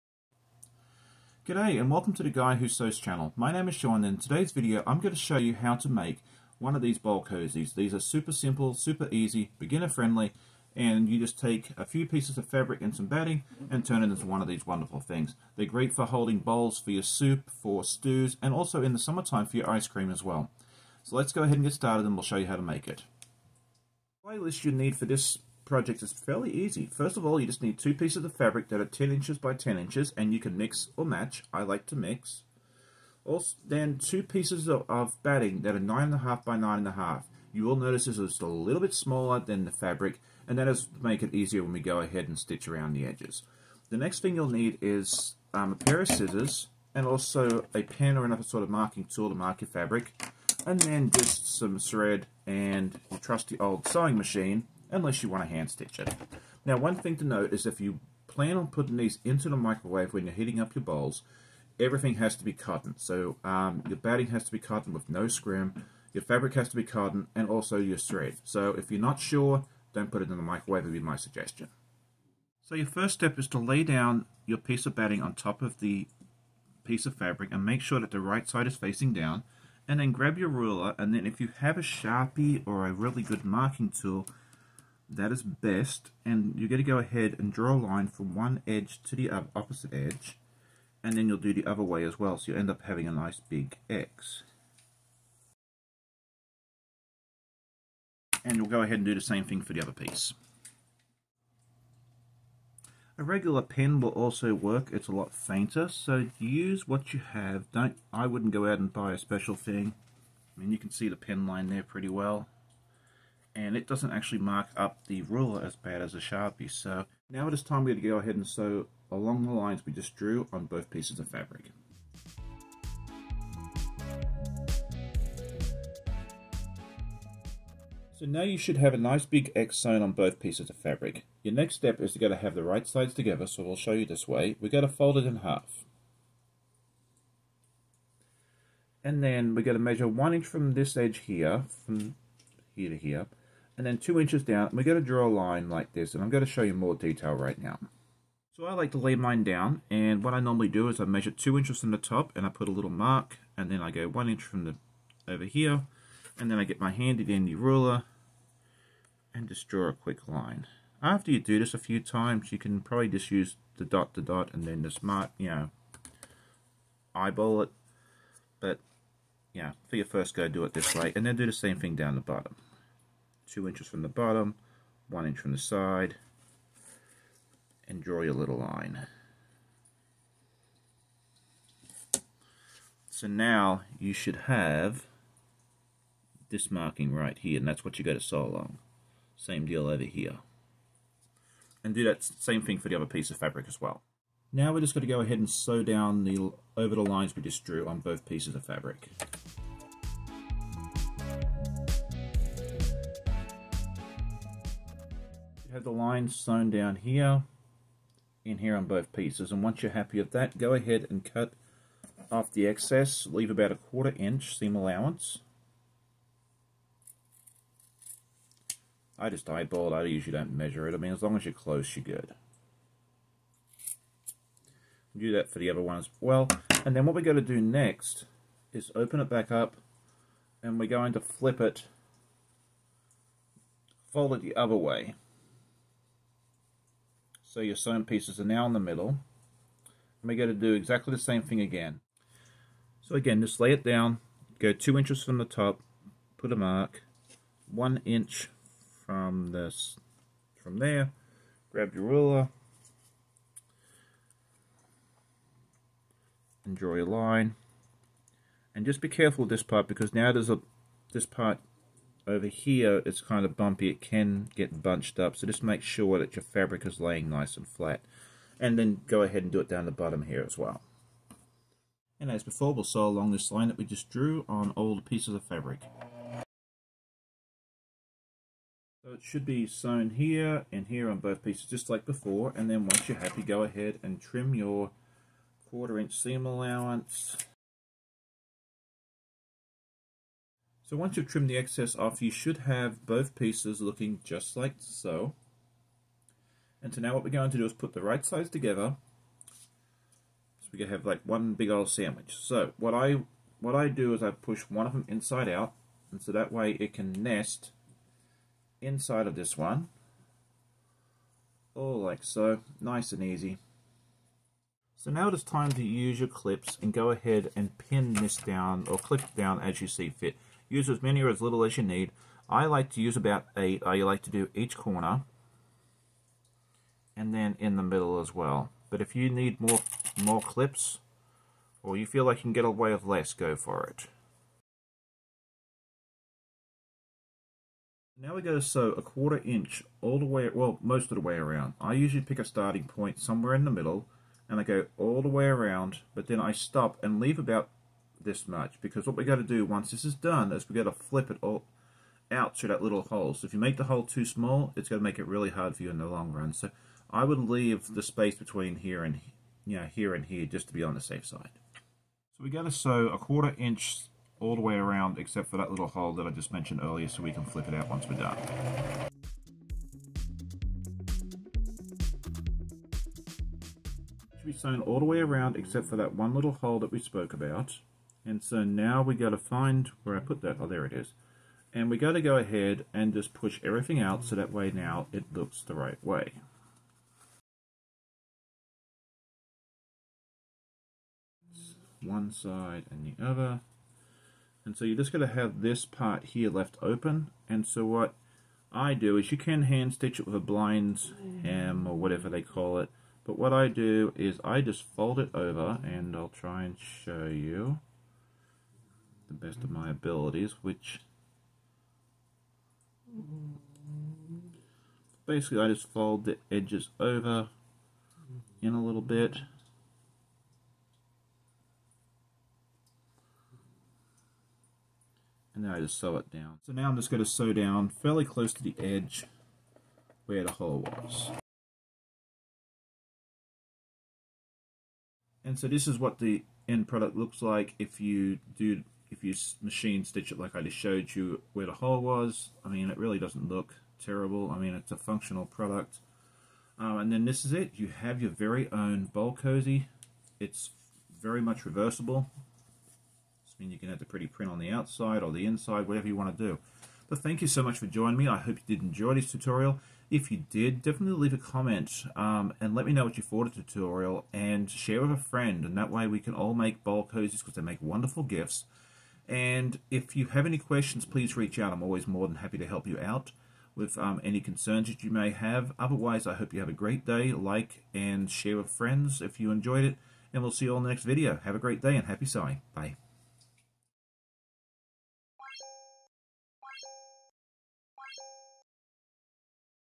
Quick and Easy Bowl Cozy Tutorial
Let's make a Bowl Cozy together. They are quick and easy to make and are one of the best gifts ive ever given.

Materials needed
2 pieces of fabric. Each piece is 10 x 10 inches
2 pieces of cotton batting. Each piece is 9.5 x 9.5 inches
Scissors
Ruler
Pen or marker
Thread
Sewing machine.

To make it look more finished you can machine stitch all the way around once you sew the hole closed.

I'd love to see photos of your completed bowl cozies.